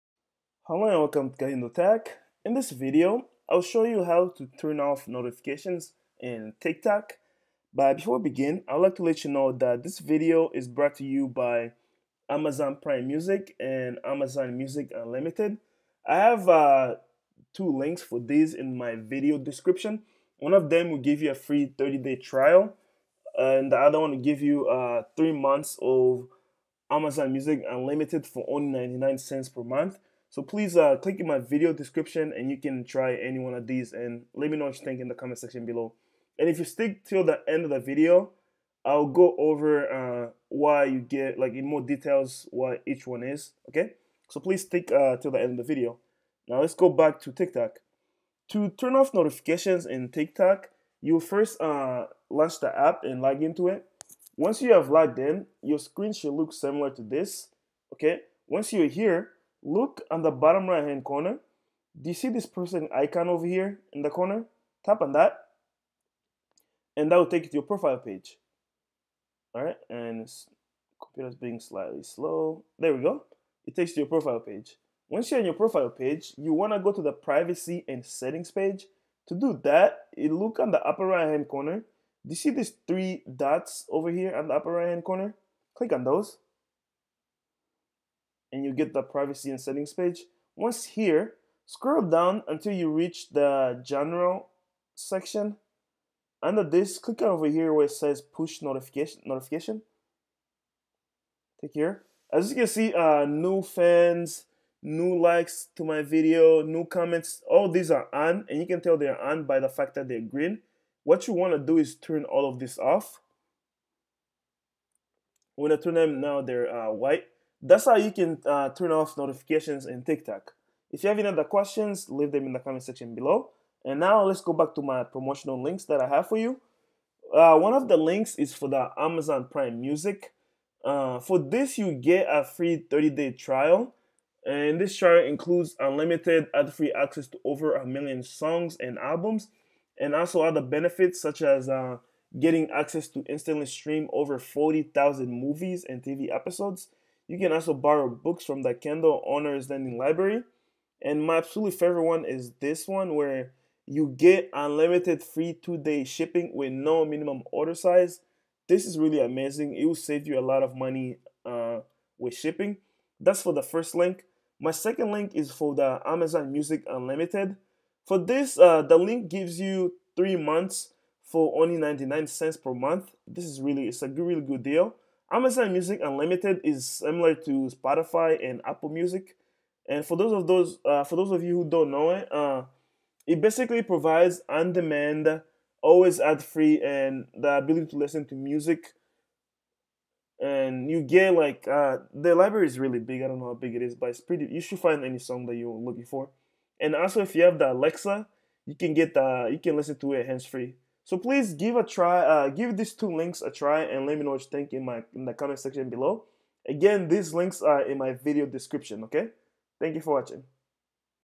How To Turn Off Notifications TikTok
This video shows How To Turn Off Notifications TikTok.

* Amazon Music Unlimited 3-Months For Only $0.99

* Amazon Prime Music Free 30-Day Trial